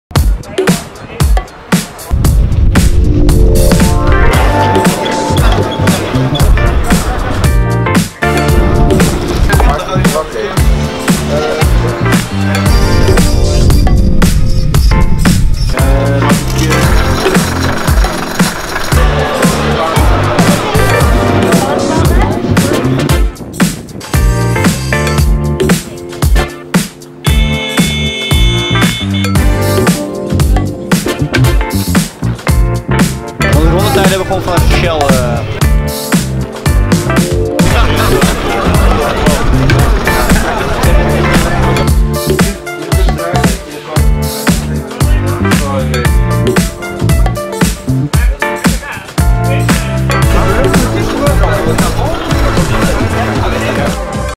A Day at Shell Eco-Marathon London 2019
A short clip of the people and the ambiance during the Shell Eco-marathon 2019, hosted at the  Mercedes-Benz World racetrack in London. Take a look in the paddock and at the track in this new video 🎬